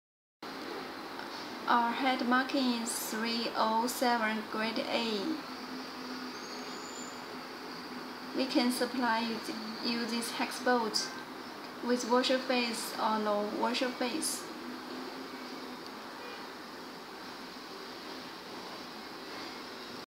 China Hex bolt Supplier-Jian & Mei
The hex bolt application in  Mechanical equipment Industry, auto mobile Industry, etc.